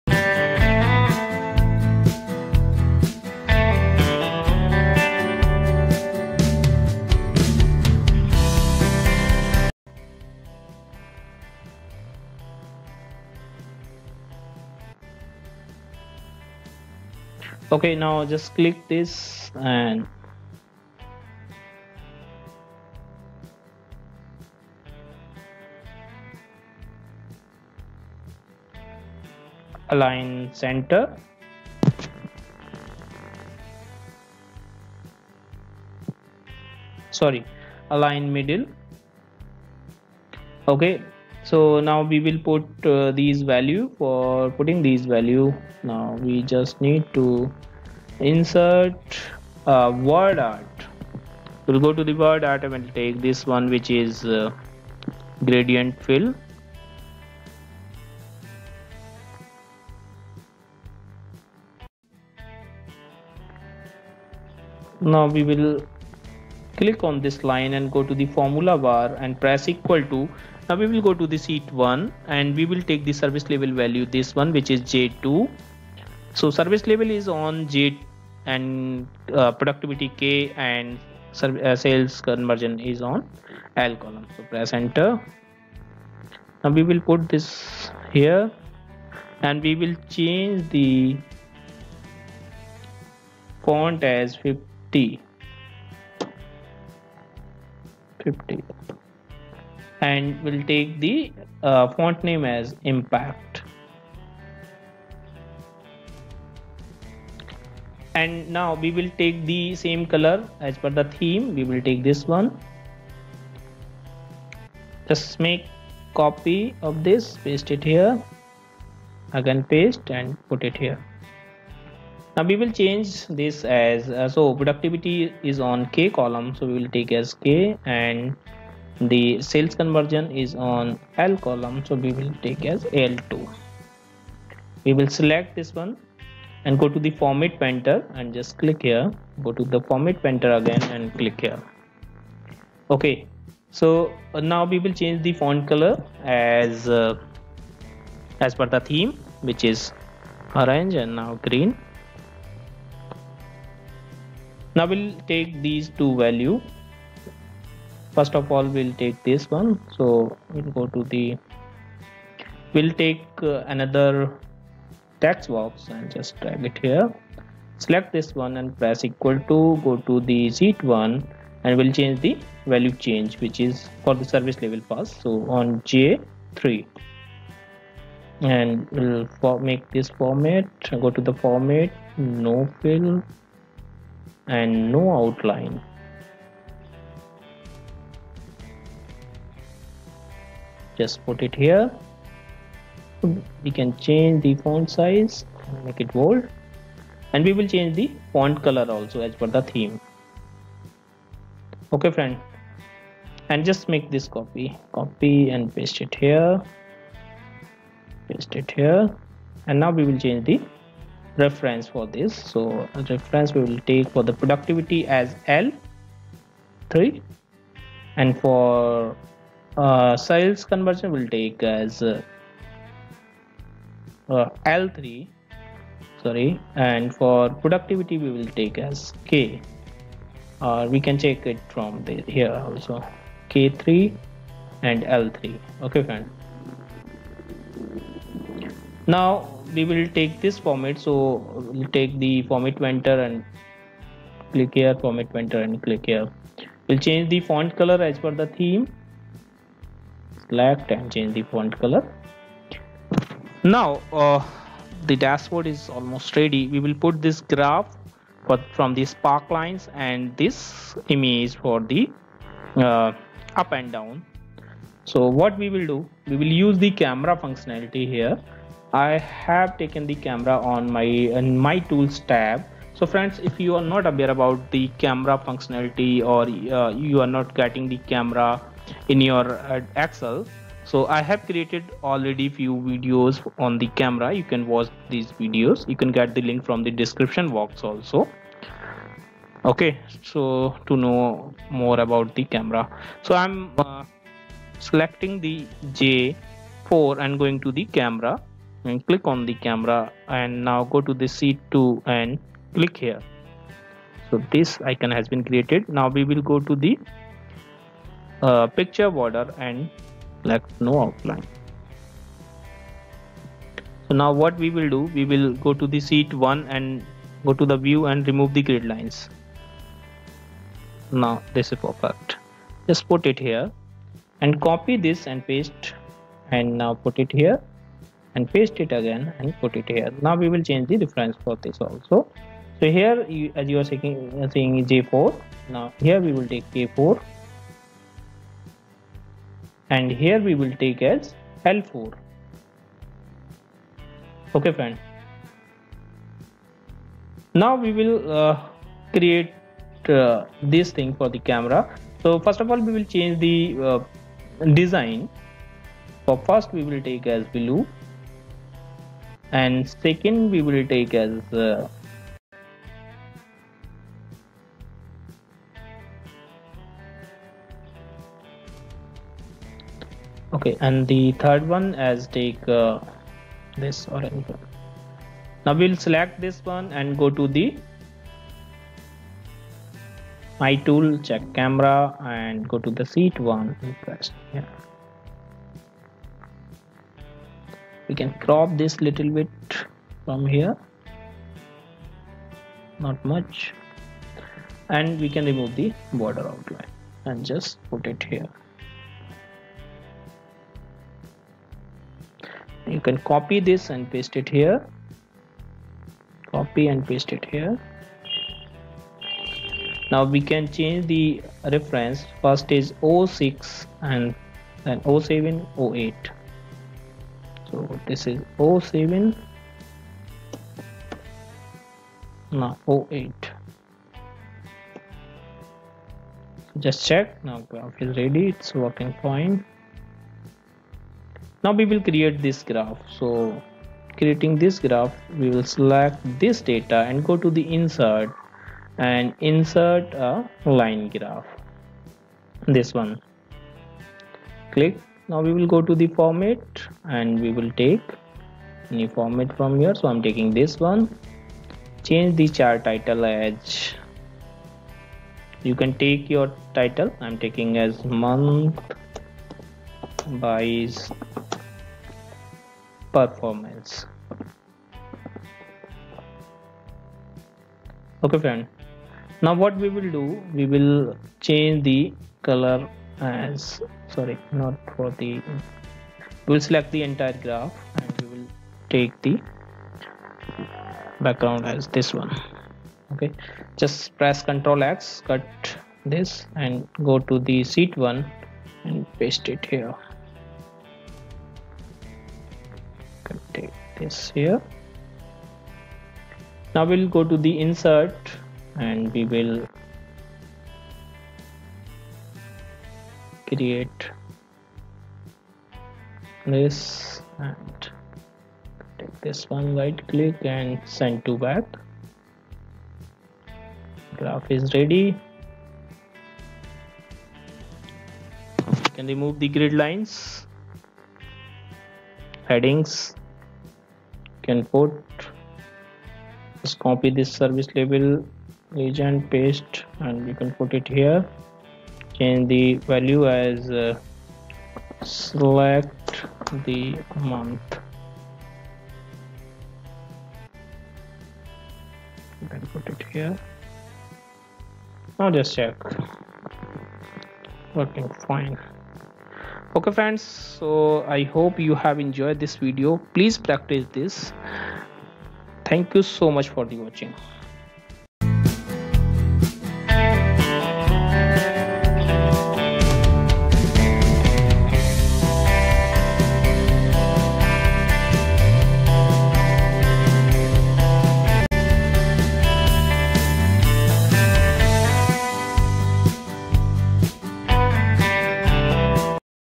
Simple Excel Dashboard (Part-2)
Hello Friends,

This is the second and last part of Simple excel dashboard. In this video you will learn how to create the cards in excel and how can you make your dashboard more attractive and beautiful by using the simple tricks.

Suggested Books
Excel Dashboard
Power Query
Power Pivot and Power BI
Exam Ref 70-778 (Power BI)